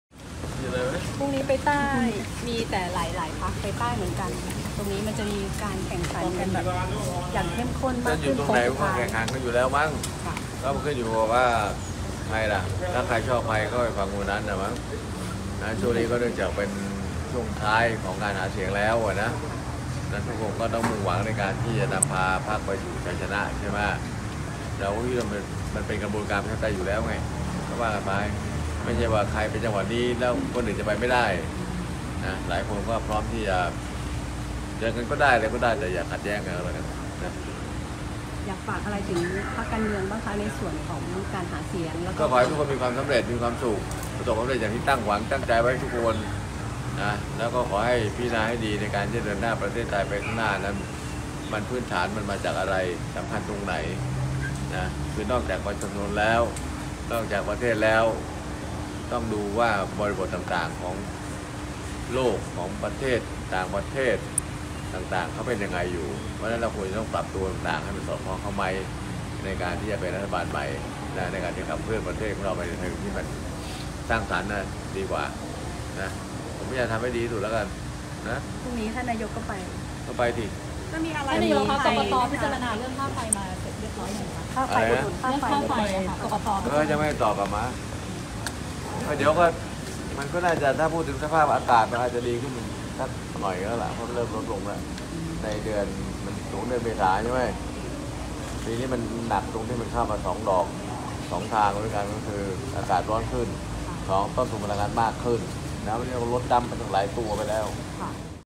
“บิ๊กตู่” ลั่นทำให้ดีที่สุด โค้งสุดท้ายเลือกตั้ง (Footage)
“บิ๊กตู่” ลั่นทำให้ดีที่สุด โค้งสุดท้ายเลือกตั้ง

28 เม.ย.2566 พล.อ.ประยุทธ์ จันทร์โอชา นายกรัฐมนตรีและรมว.กลาโหม ไม่หวั่นหลายพรรครุมทึ้งสนามใต้ พร้อมเจอทุกพรรค แต่อย่าขัดแย้ง เผย กกต.ยังไม่ตอบอนุมัติงบกลางหมื่นล้านช่วยค่าไฟชาวบ้าน